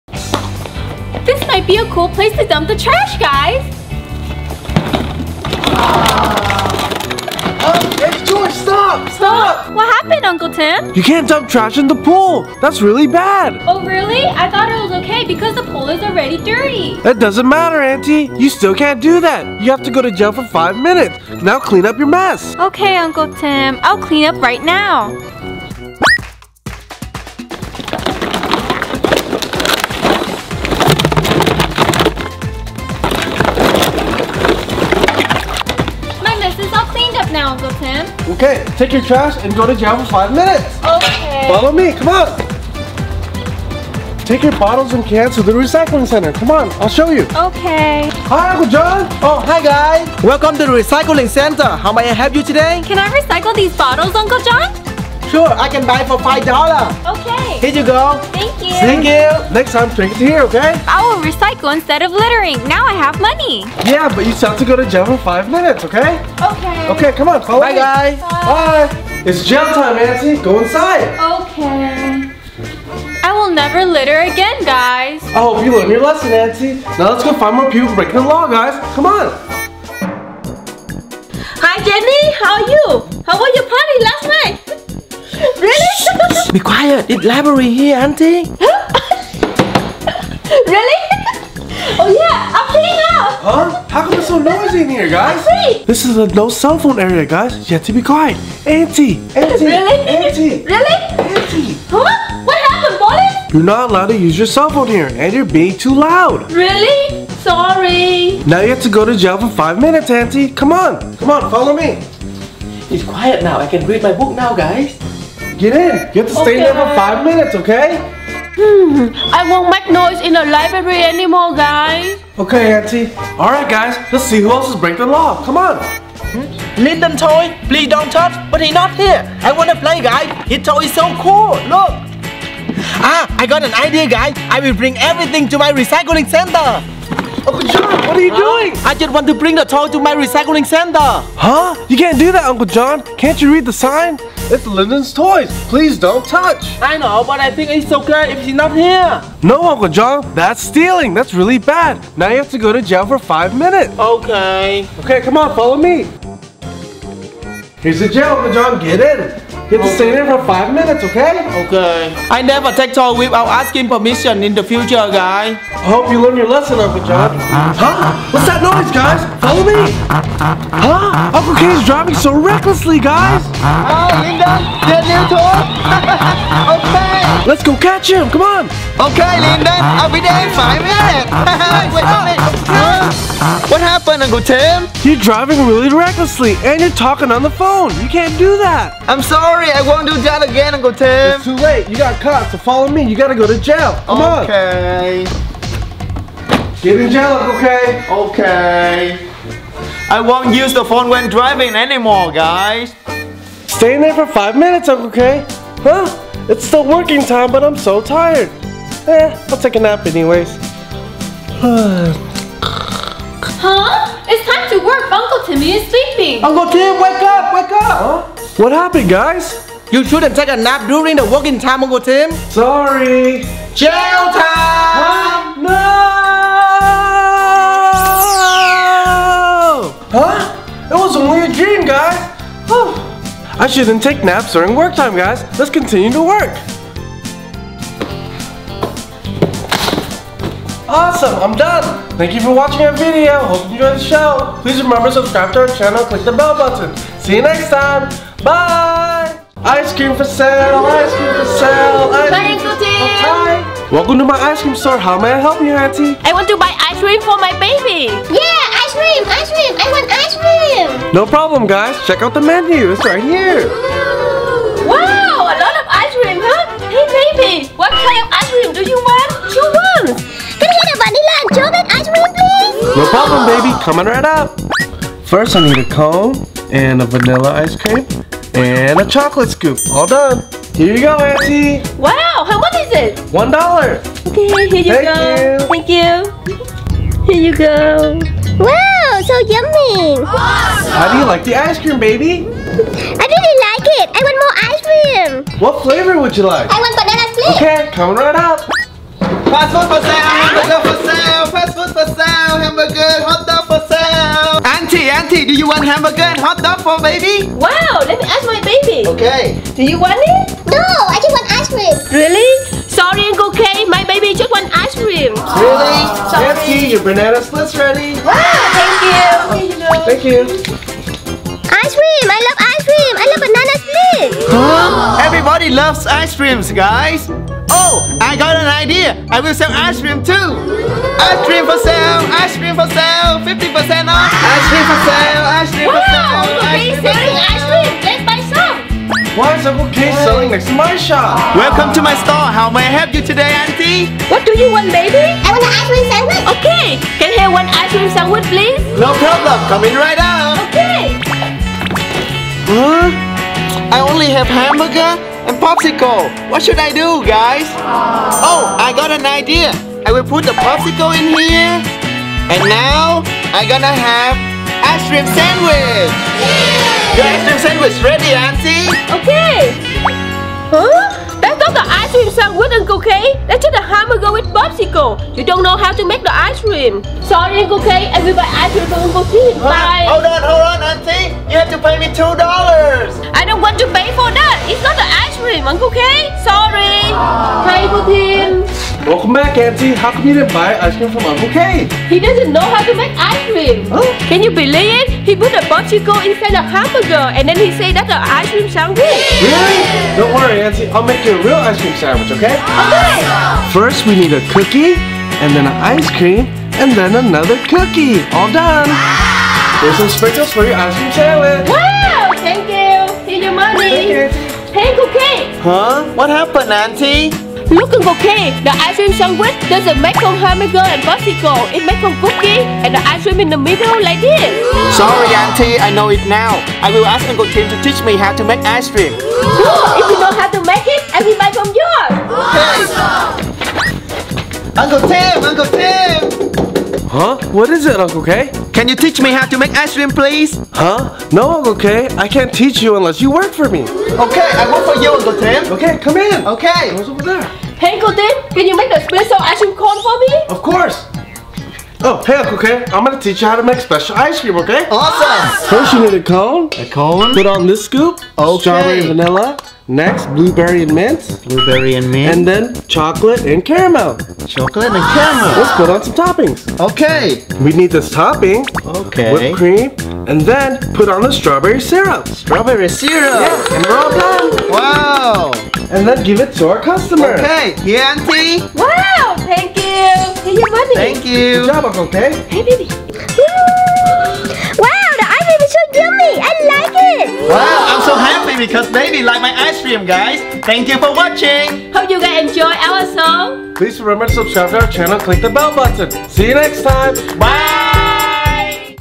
Funny Aunties & Uncle Gets LOCKED UP in Pretend Jail
Funny Auntie Joyce gets locked up in a pretend jail by police officer Uncle Tim. Auntie Joyce was caught dumping trash into the swimming pool by cop Uncle Tim and was sent to jail. Uncle Tim showed her that she can take her recycled bottles to the recycling center instead of dumping them. Next, Auntie Rose was talking really loud in a no cell phone zone and had to be taken to jail. Kids learn they need to keep a quiet voice in places like the library. Uncle John started to play with Lyndon’s toys without permission, so police Uncle Tim took him to jail to teach him that is wrong. Kids learn proper behavior on how to act in their daily lives in a fun way!